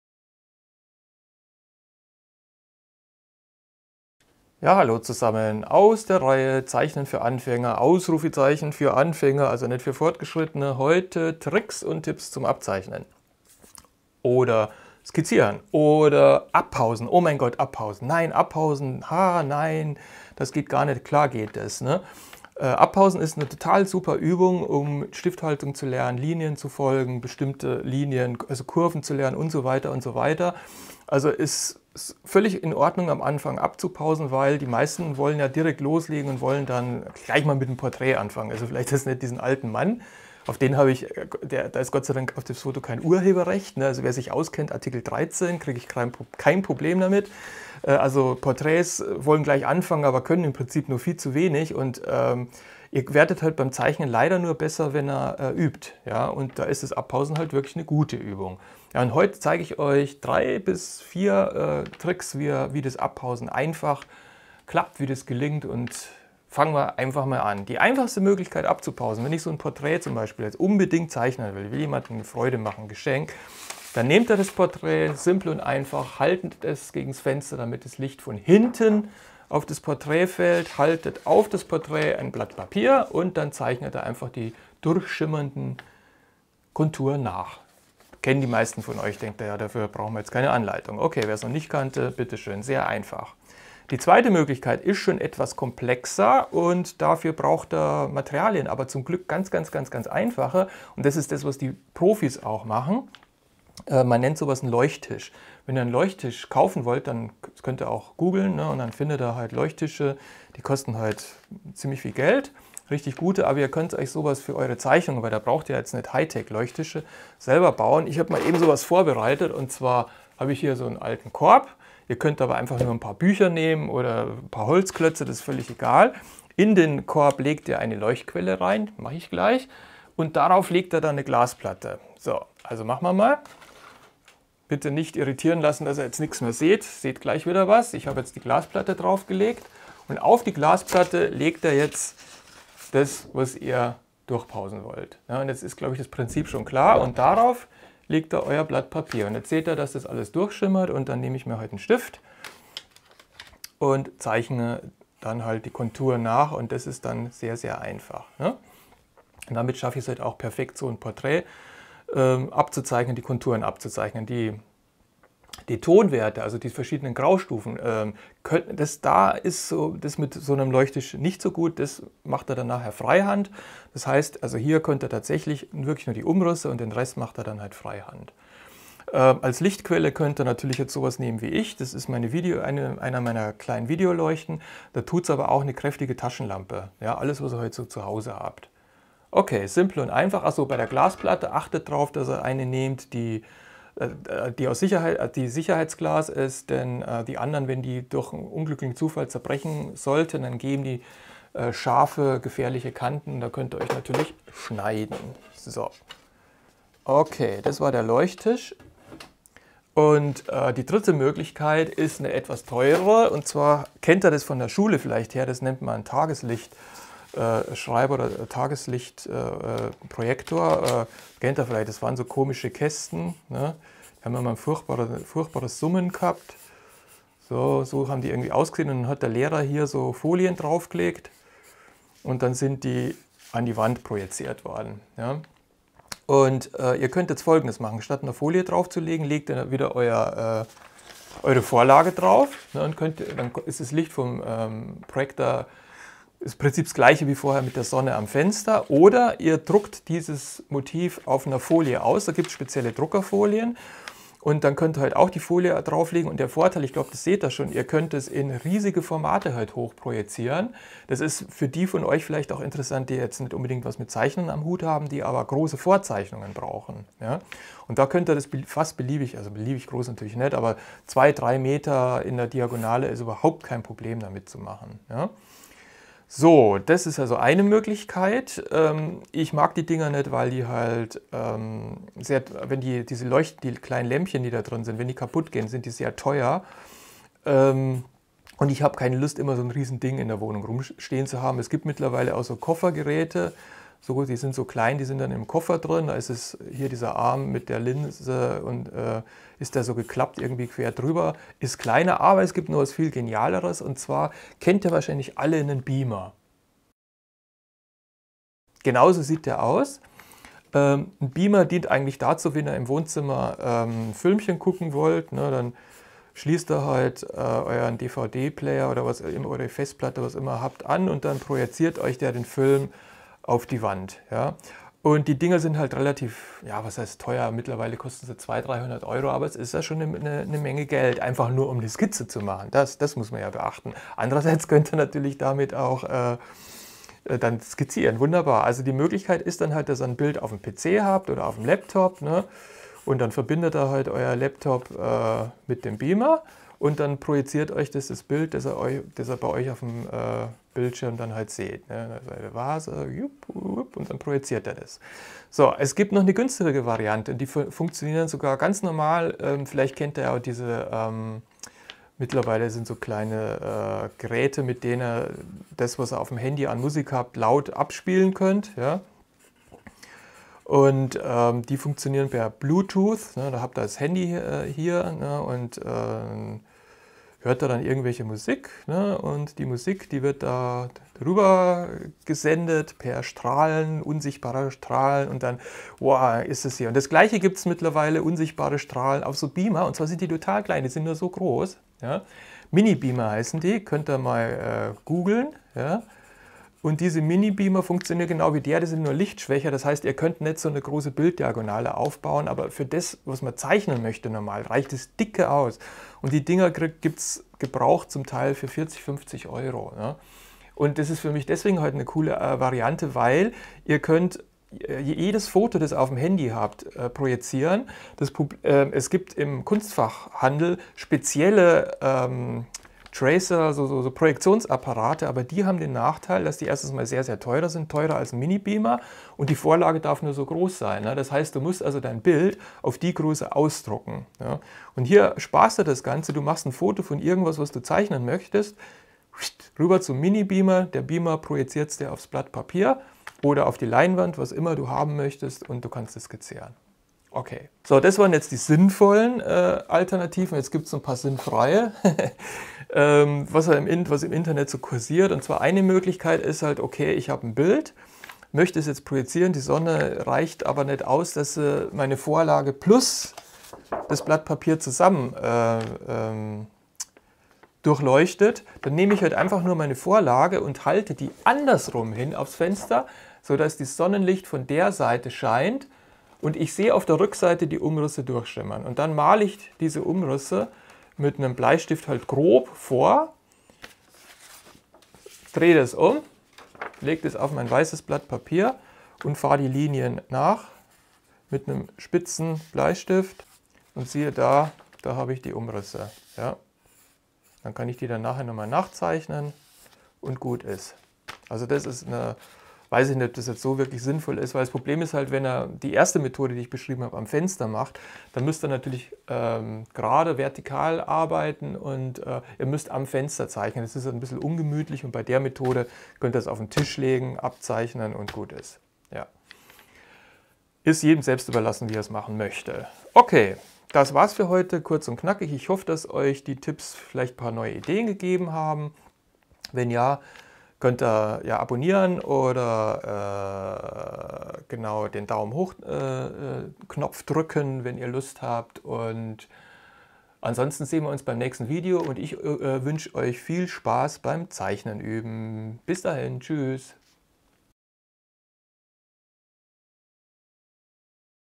Zeichnen für Anfänger (6): Bildübertragung
Heute zeige ich euch ein paar sehr einfache und hilfreiche Tricks, wie ihr Bilder "abpausen" könnt.
Mehr Infos zu meiner Kunst, meinen Malbüchern und Malkursen findet ihr